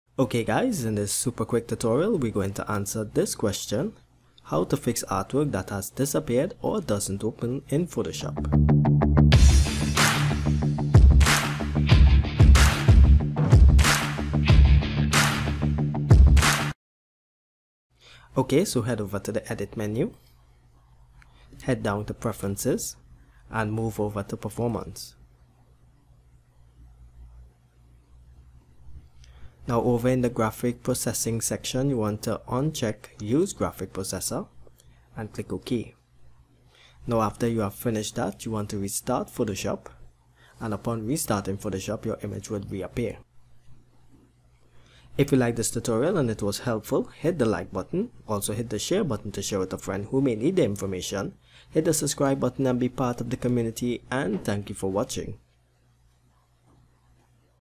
Adobe: Photoshop Troubleshooting Image or Artwork Disappears/Doesn't Display in Photoshop
In this tutorial we solve the issue of images or artwork not displaying of disappearing during a session.